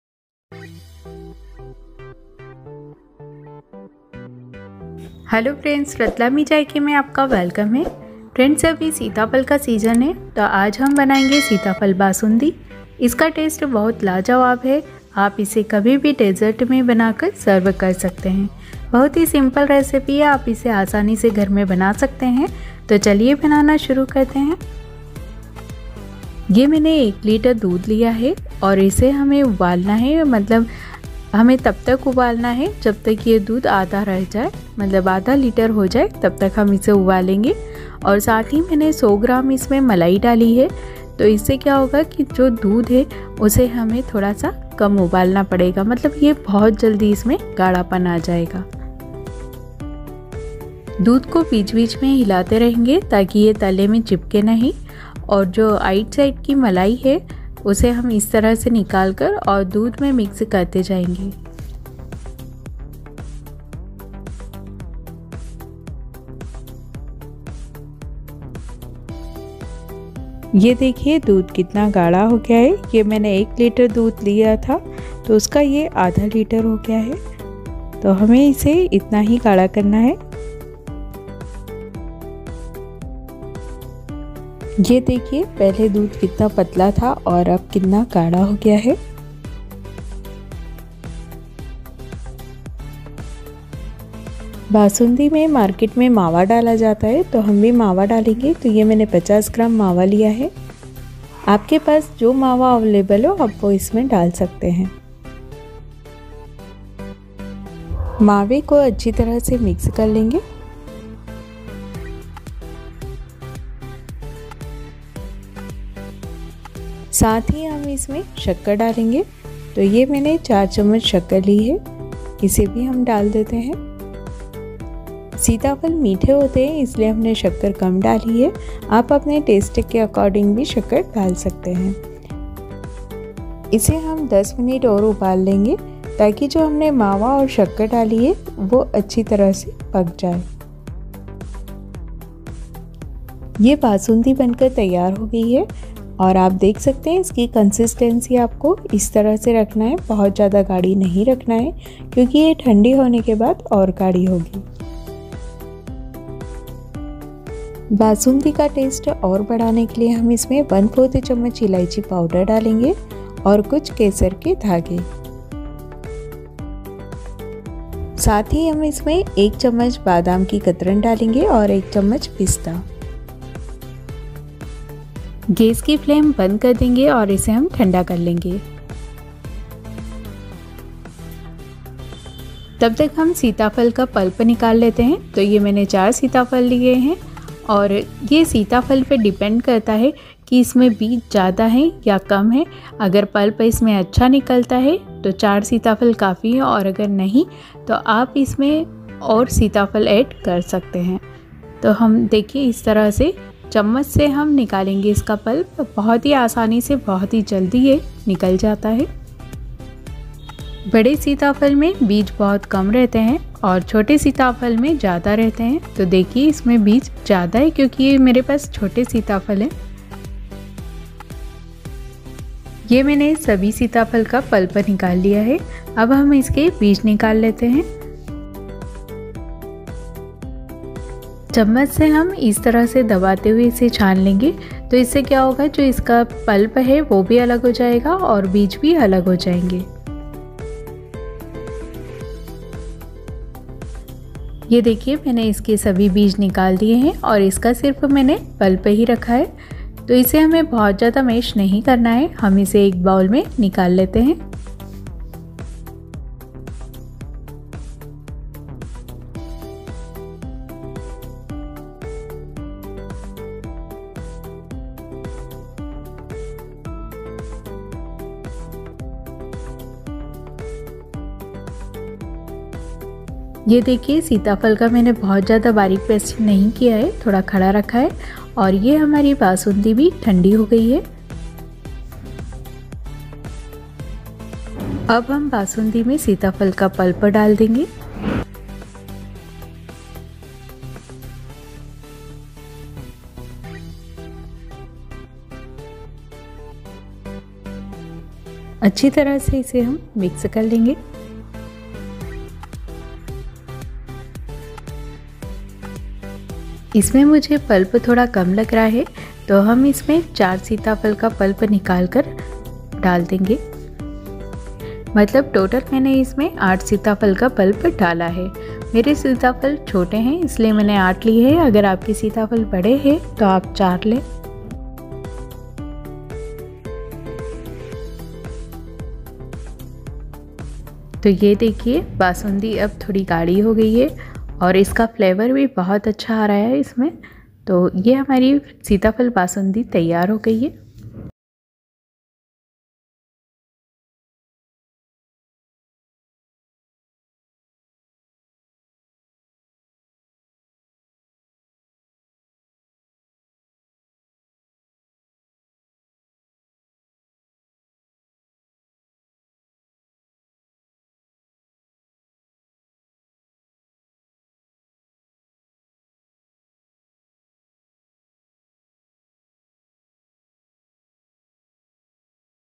सीताफल बासुंदी बनाने का तरीका,Home Made Sitafal Basundi,Basoondi Recipe,Ratlami Zayka,Sonal Pathak
आज हम बनाएंगे सीताफल बासुंदी इसका टेस्ट बहुत ही लाजवाब है आप इसे कभी भी डेजर्ट में बनाकर सर्व कर सकते हैं बहुत ही सिंपल रेसिपी आप आसानी से घर में बना सकते हैं !